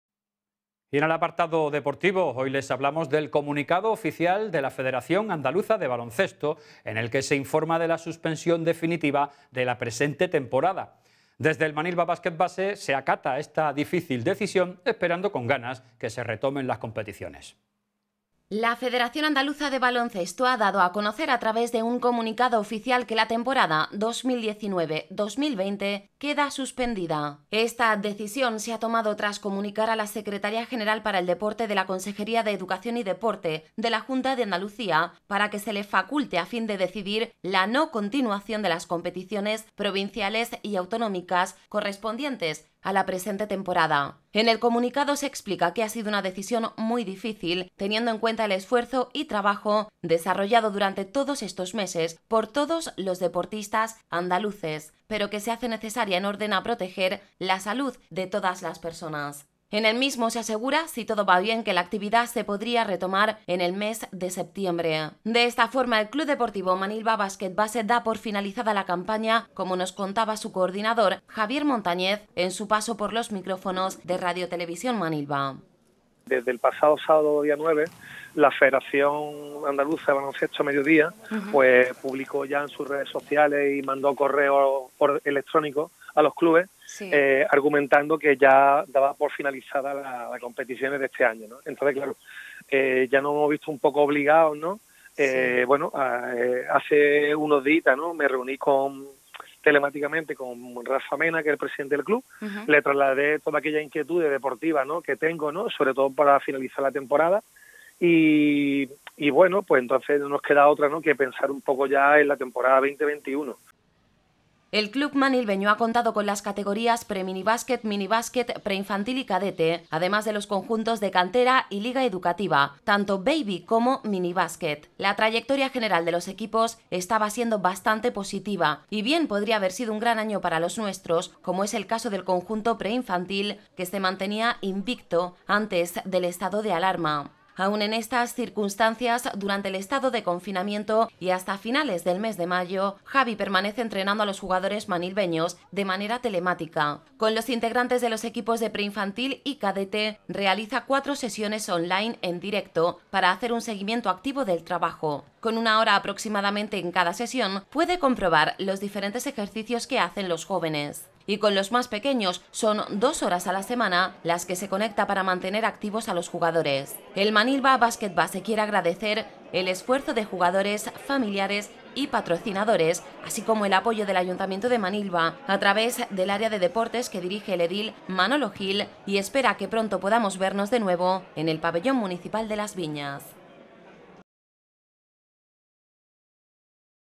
Suspendida la temporada de baloncesto
La Federación Andaluza de Baloncesto ha dado a conocer a través de un comunicado oficial que la temporada 2019/20 queda suspendida.
Esta decisión se ha tomado tras comunicar a la Secretaria General para el deporte de la Consejería de Educación y Deporte de la Junta de Andalucía para que se le faculte a fin de decidir la no continuación de las competiciones provinciales y autonómicas correspondientes a la presente temporada.
En el comunicado se explica que ha sido una decisión muy difícil, teniendo en cuenta el esfuerzo y trabajo desarrollado durante todos estos meses por todos los deportistas andaluces pero que se hace necesaria en orden a proteger la salud de todas las personas. En el mismo se augura, si todo va bien, que la actividad se podría retomar en el mes de septiembre.
De esta forma, el CD Manilva Basket Base da por finalizada la campaña como nos contaba su coordinador, Javier Montañez, en su paso por los micrófonos de RTV Manilva.
El Club manilveño ha contado con las categorías preminibasket, minibasket, preinfantil y cadete, además de los conjuntos de cantera y Liga Educativa, tanto baby como minibasket. La trayectoria general de los equipos estaba siendo bastante positiva, y bien podría haber sido un gran año para los nuestros, como es el caso del conjunto preinfantil, que se mantenía invicto antes del estado de alarma.
Aún en estas circunstancias, durante el estado de confinamiento y hasta finales del mes de mayo, Javi permanece entrenando a los jugadores manilveños de manera telemática.
Con los integrantes de los equipos de preinfantil y cadete realiza cuatro sesiones on-line en directo, para hacer un seguimiento activo del trabajo. Con una hora aproximadamente en cada sesión, puede comprobar los diferentes ejercicios que hacen los jóvenes.
Y con los más pequeños, son dos horas a la semana las que se conecta para mantener activos a los jugadores.
El Manilva Basketbase, quiere agradecer el esfuerzo de jugadores, familiares y patrocinadores, así como el apoyo del Ayuntamiento de Manilva, a través del área de Deportes, que dirige el edil Manolo Gil, y espera que pronto podamos vernos de nuevo en el Pabellón Municipal de Las Viñas.